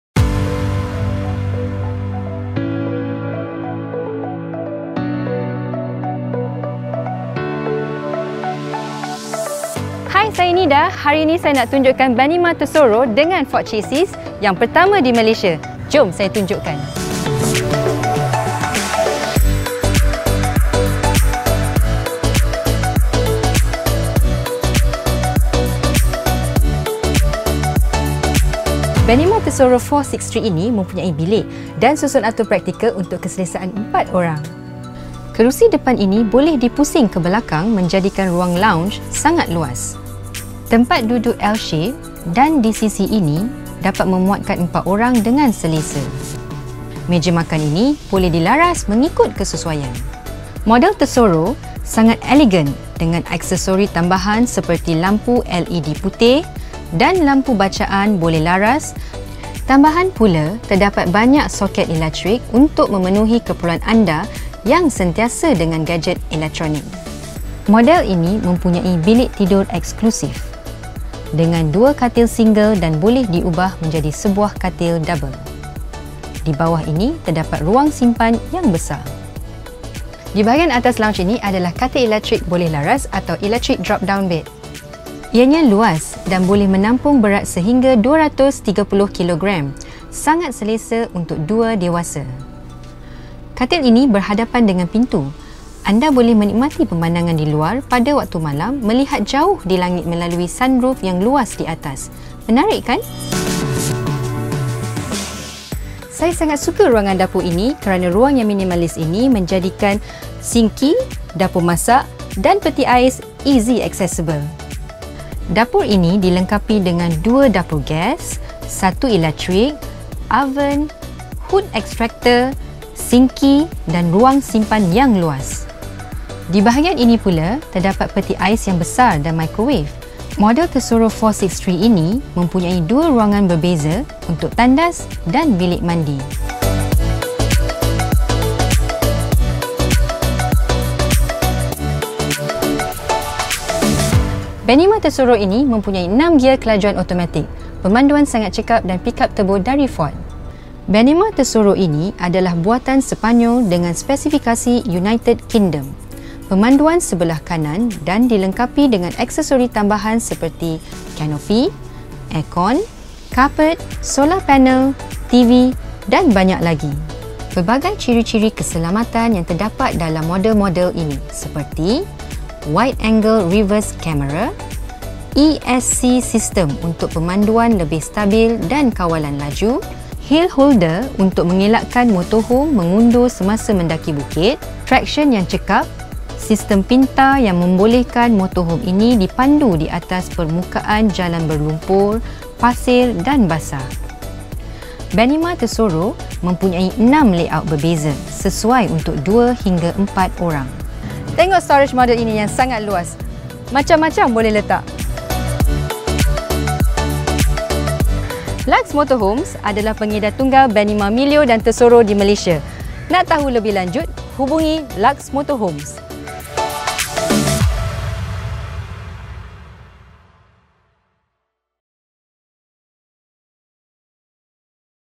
BENIMAR TESSORO 463 - FORD CHASIS MOTORHOME FIRST IN MALAYSIA
Luxe Motorhomes & Caravans is proud to introduce Benimar Tessoro 463 - the FIRST FORD CHASIS Motorhome in Malaysia.

The twin bed 463 model is practical for a family of four. The two rear beds that can be converted into a huge double are ideal for families and couples who are sometimes accompanied.

There is also a drop down double bed above the dining room/lounge for two more.

A front dinette area is complimented with rotating cab captain style seats and provides very comfortable eating for up to 5 people.

The bathroom is separated into two and the rear bathroom/bedroom area has a privacy door which separates it from the living room.

There is also a large garage underneath which is accessible from both sides.

to see more ranges & price.